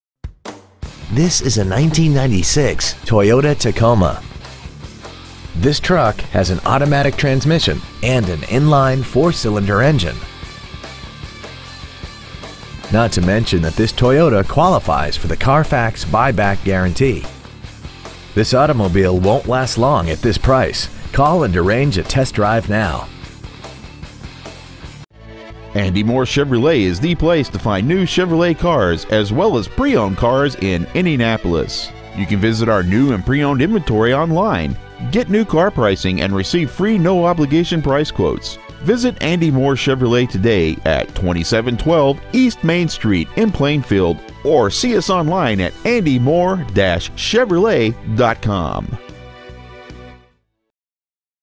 1996 Toyota Tacoma Indianapolis IN 46168
Year : 1996
 Make : Toyota
 Model : Tacoma
 Engine : 2.4L I4 16V MPFI DOHC
 Trans . : Automatic
 Exterior : Black
 Miles : 139,792
 Interior : Gray
 Stock : PC2316A

 2712 E. Main Street